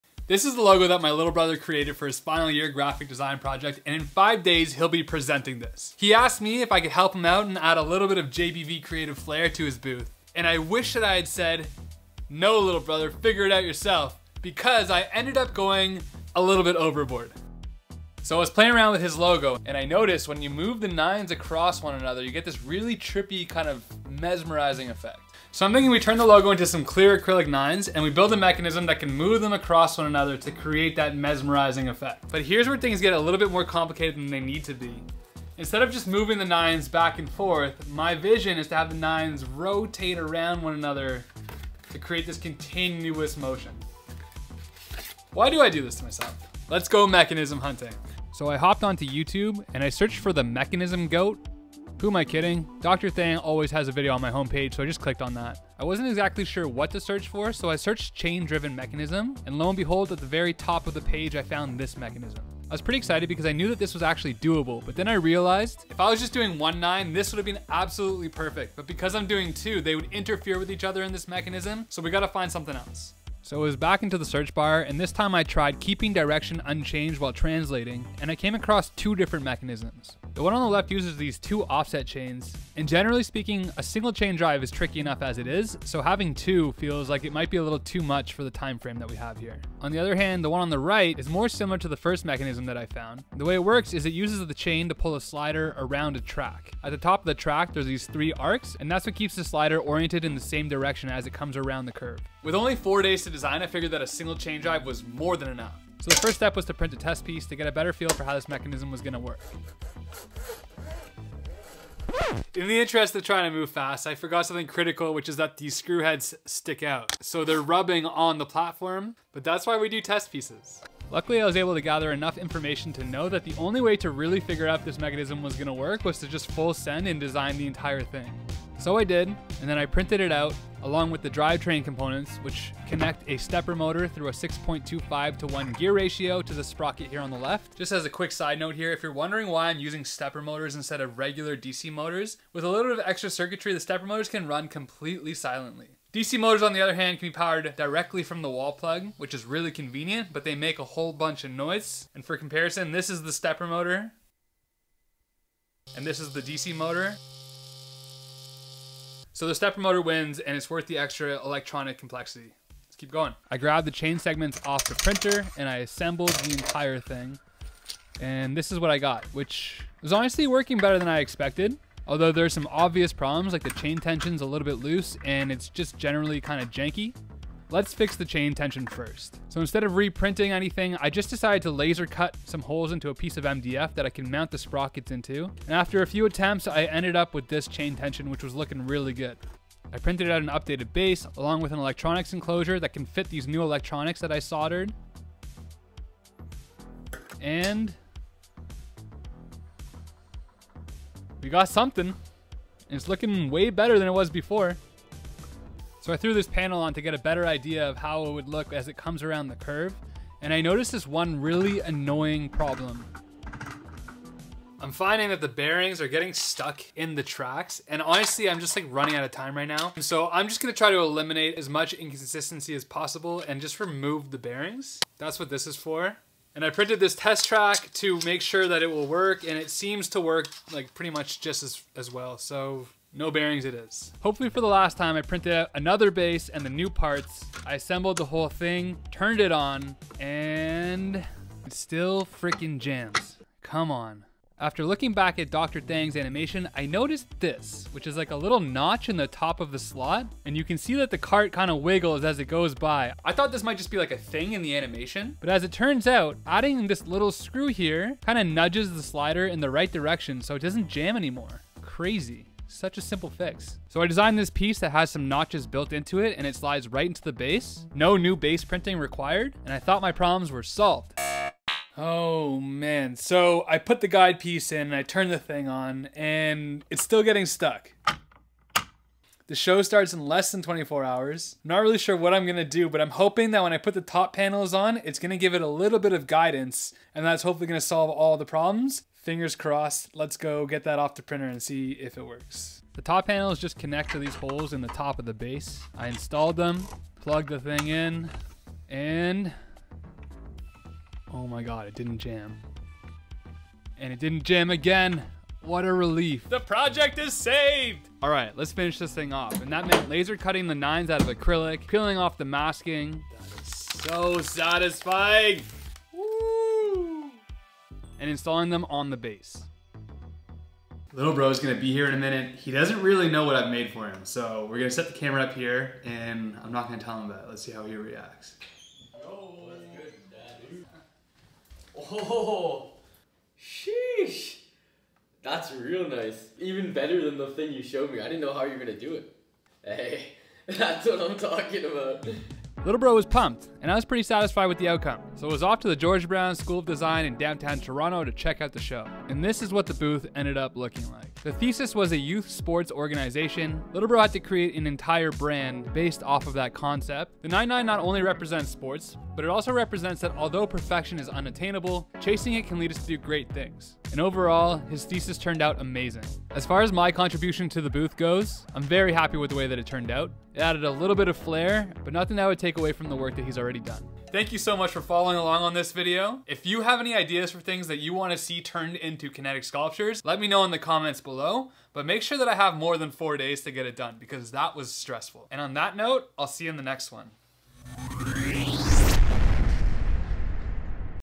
Making a Digital Logo Move IRL
Become an Engineezy Thangs Member and get access to 2+ premium files every month

My little brother asked me if I can help him out with his final graphic design thesis presentation.  I agreed, and then went a little overboard, designing, 3D printing, and building a fully functioning kinetic sculpture...all in 4 days.

The mechanism was based off of this from @thang010146

And features a chain and sprocket that pulls a cart around a slotted track.  The 9's from the logo would be connected to the cart, and follow the path of chain. My vision was for the 9s to remain in the same orientation across the entire track.  This is where the clever 3 slot design comes into effect.

After some setbacks and challenges, I was able to create a kinetic sculpture using 3D printed and laser cut parts that accentuated little bro's brand without taking away from the incredible work he accomplished on this project.  Win win for both of us!